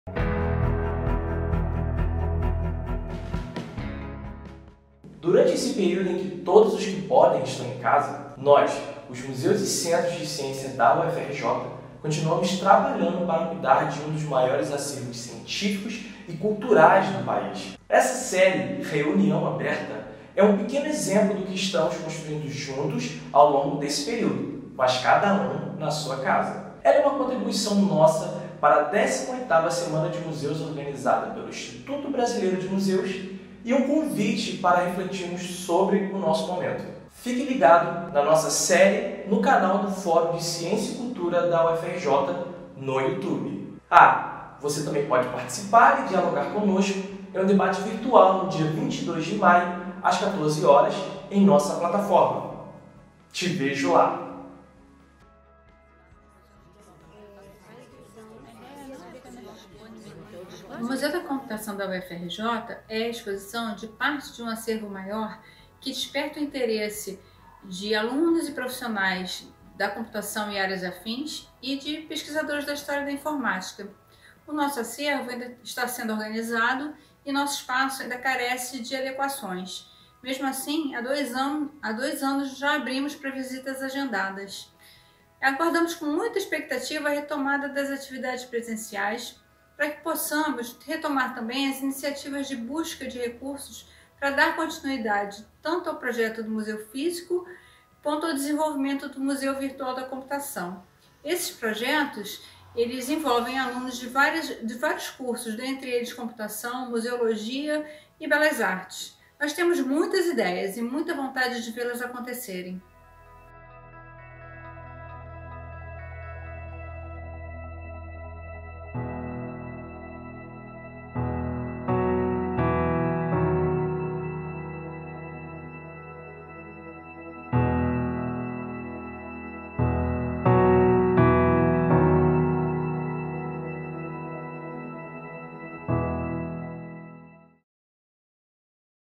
Museu da Computação - Reunião Aberta! Museus da UFRJ
Os Museus da UFRJ estão fechados ao público no momento, mas seus servidores continuam trabalhando remotamente para garantir que o retorno de atividades presenciais seja o melhor possível para a comunidade universitária e a sociedade brasileira. Temos muitas expectativas sobre como vamos adotar novas abordagens físicas e comunicacionais para interagir com o público nesse mundo novo.

Acompanhe um pouco do que estamos debatendo na nossa série ReuniãoAberta! no youtube. E venha debater conosco em nosso encontro virtual no dia 22 de maio, às 14:30 pela plataforma

A Reunião Aberta é uma atividade do SIMAP (Sistema de Museus, Acervos e Patrimônio Cultural da UFRJ), dentro da Semana de Museus promovida pelo Instituto Brasileiro de Museus - IBRAM, entre os dias 18 e 21/5. O SIMAP é um dos órgãos suplementares do Fórum, instituído em 2018 com o objetivo de coordenar as políticas de recuperação e preservação dos acervos da UFRJ.